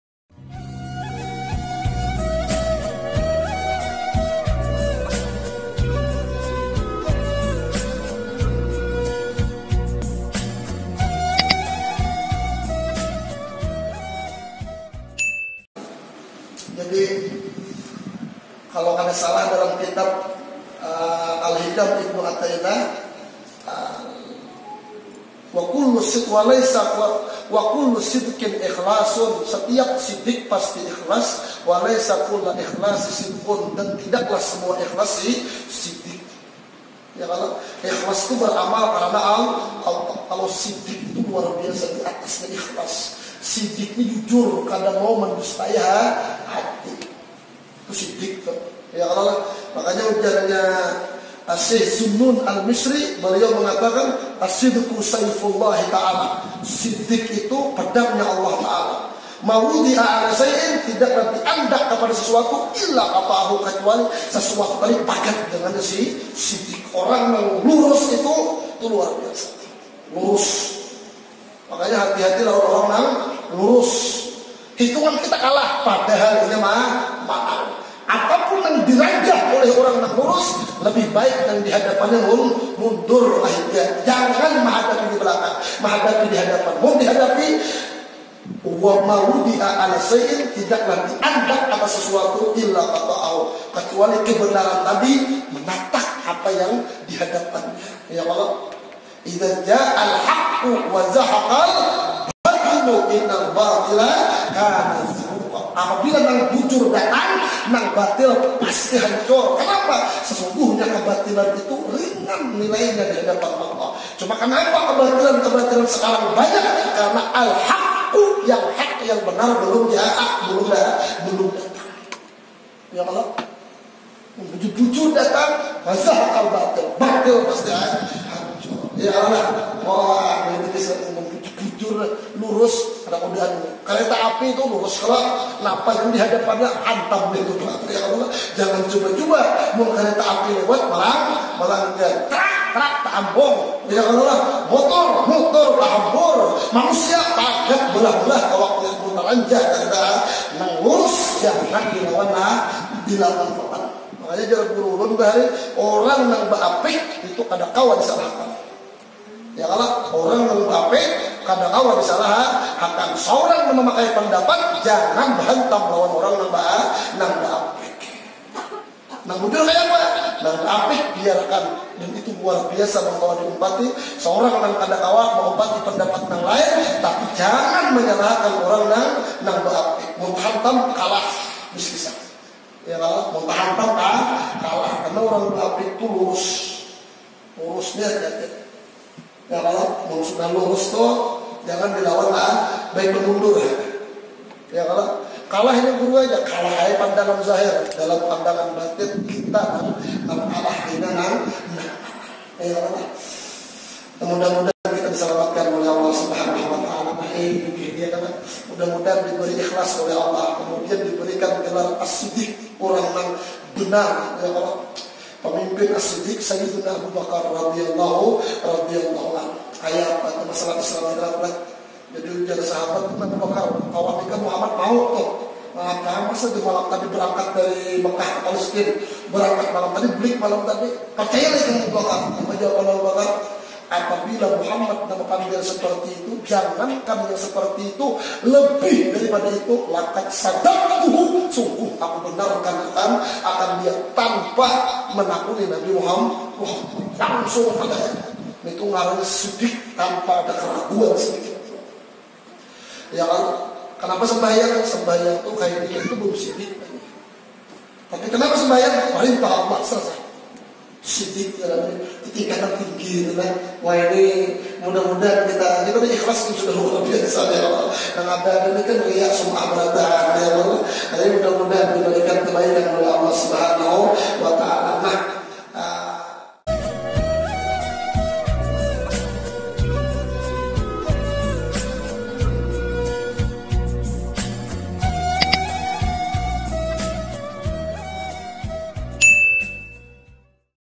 MAJELIS RUTIN MALAM RABU : LEBIH TINGGI MANA SIFAT IKLAS DENGAN SIDDIQ ? || MT. AR-RIYADH TANBU
Kajian Kitab: Fawaidul Mukhtaroh (Al-Habib Zain Bin Smith)
Waktu : Selasa Malam (Malam Rabu)
Tempat : Jl.Remaja (Sebrang Masjid Baiturrahim Simpang Empat)
Batulicin,Kab.Tanah Bumbu,Kal-Sel

Dari Abu Mas’ud Uqbah bin Amir Al Anshari radhiyallahu ‘anhu, ia berkata bahwa Rasulullah shallallahu ‘alaihi wa sallam bersabda,